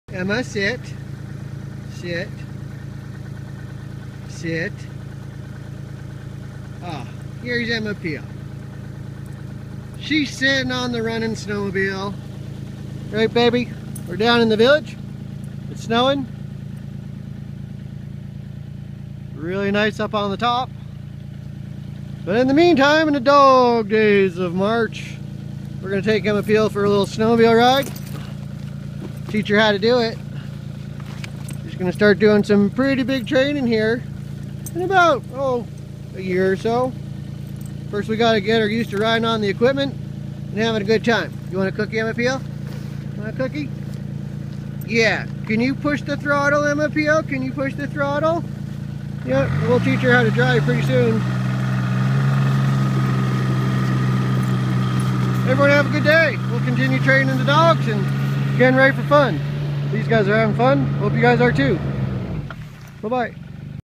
Training A Puppy to Ride a Snowmobile
Training my 5 month Labrador Retriever to ride a snowmobile.